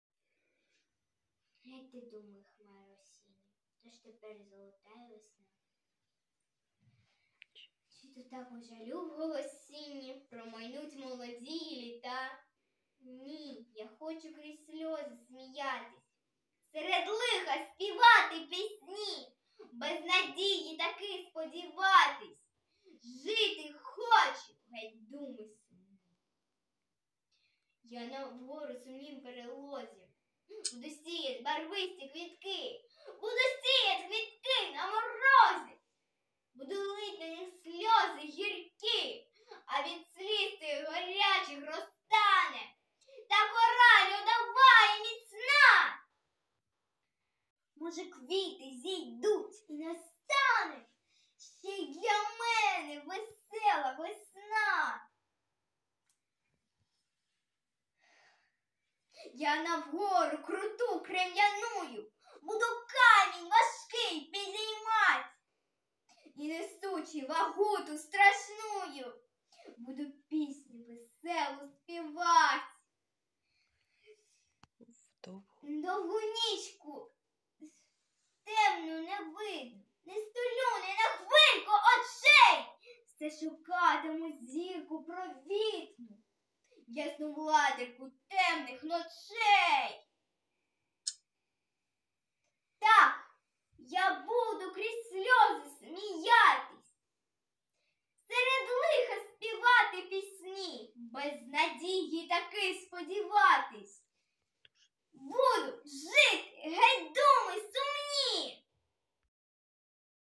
Склярова Ангеліна "contra spen spero!" | Конкурс «Змагаймось за нове життя!» 2024 рік
Номінація

Склярова Ангеліна
"contra spen spero!"

Підготувала - Ягодзинська Наталя Вікторівна вчитель навчальних клсів

Заклад - ЗОШ 3 ОМР Сумської області

* Родини Бурбак.